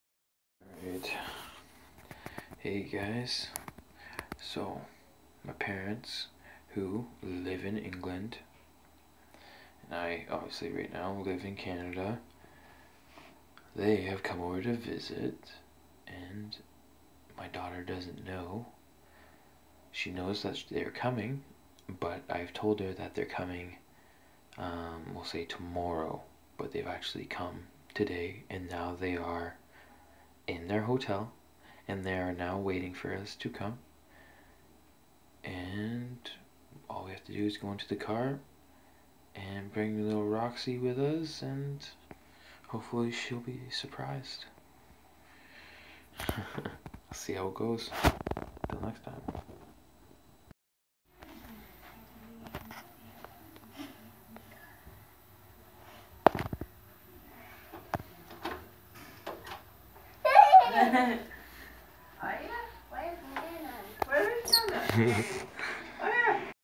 Big surprise for Daughter
Big surprise for my daughter my parents are coming and she does not know there here she thinks they are coming tomorrow.